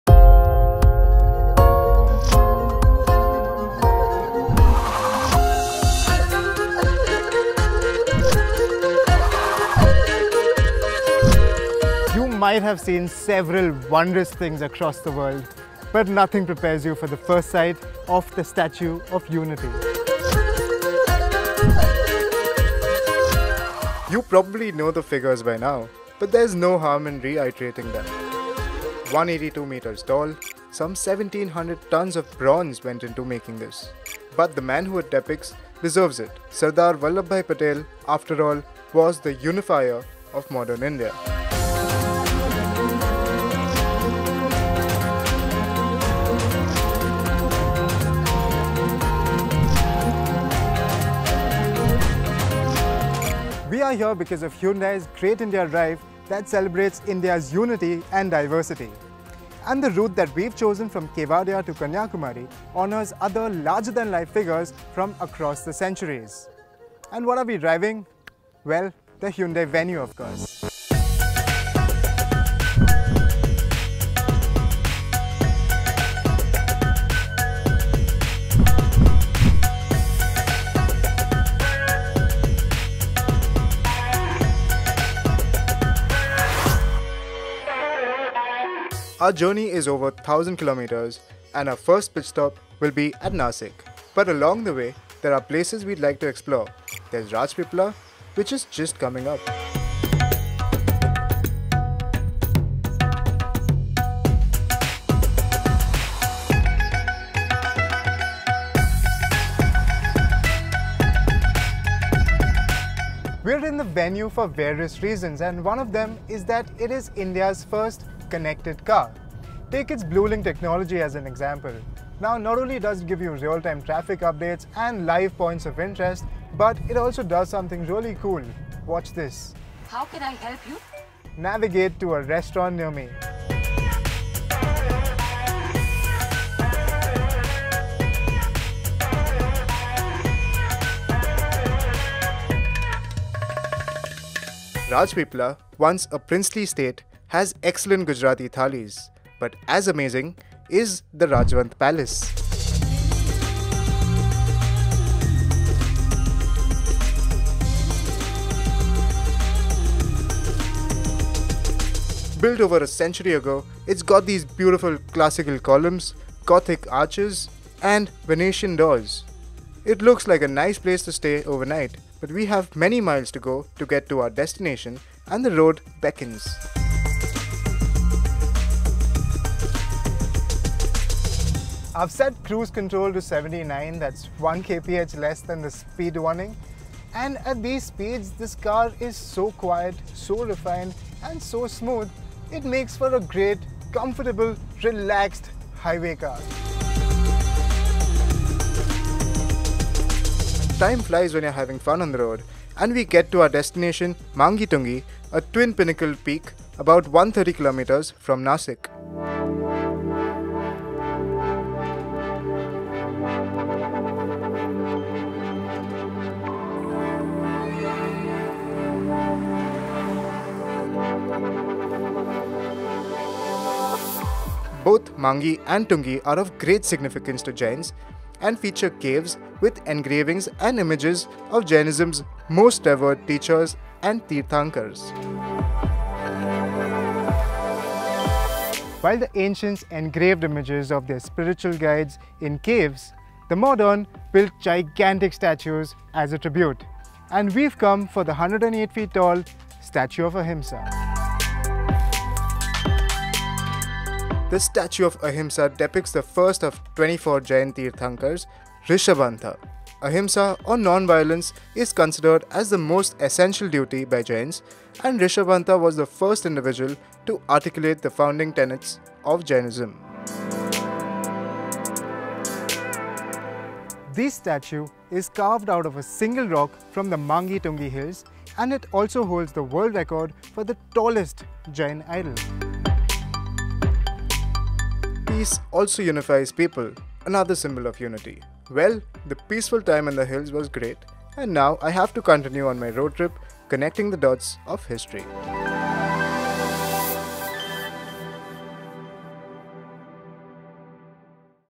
Pedal to Pedestal : Part 1 | Great India Drive with Hyundai Venue | Sponsored Feature
The Statue of Unity, the tallest statue in the world, is a tribute to Sardar Vallabhai Patel and the values he stood for. In that spirit we embark on a journey across the country on a Great India Drive with the Hyundai Venue, and our first destination is another towering statue.